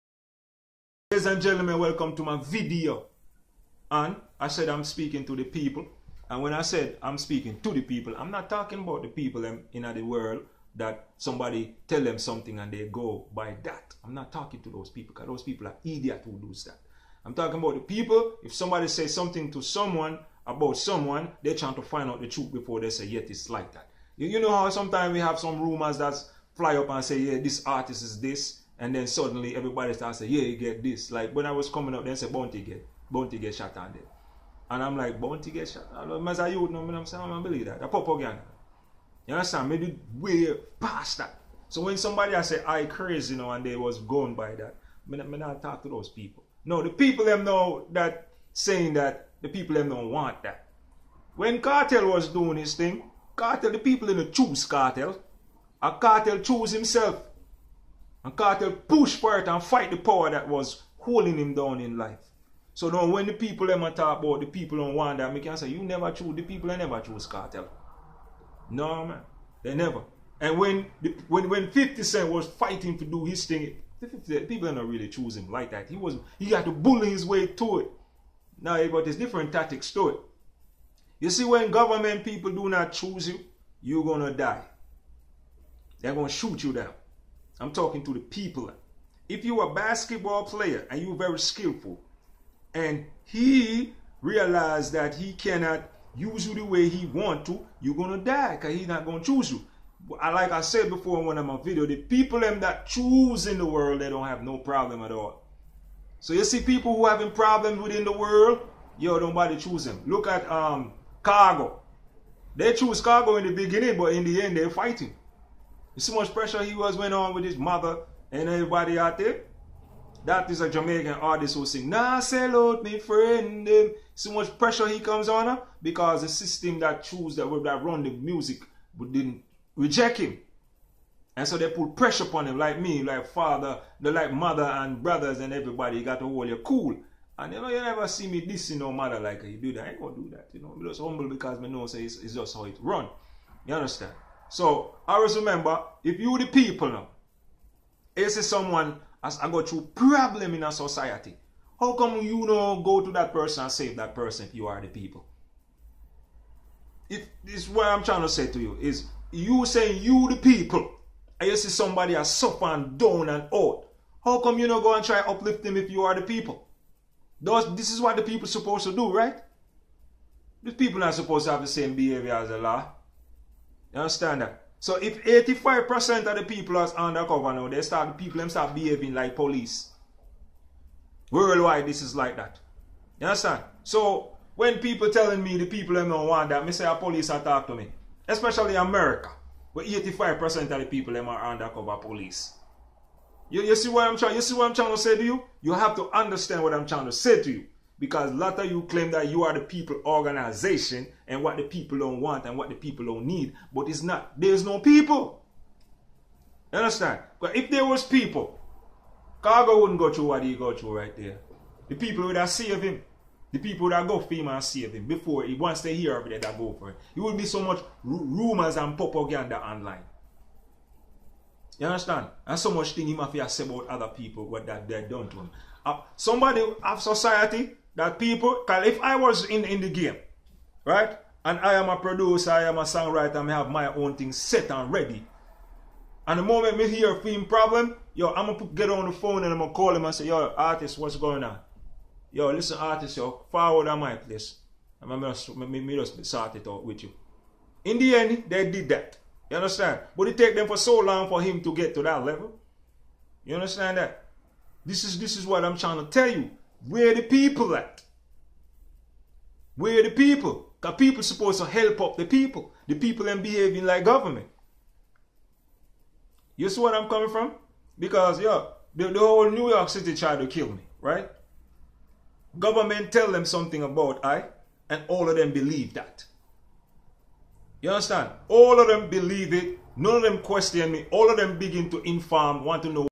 Message To The People Speaking with Understanding
Understanding Have a lot to do with living in the real world of confusion when you speaking to the peoples about anything, it is up to them to find the fact. You can't just jump to conclusions.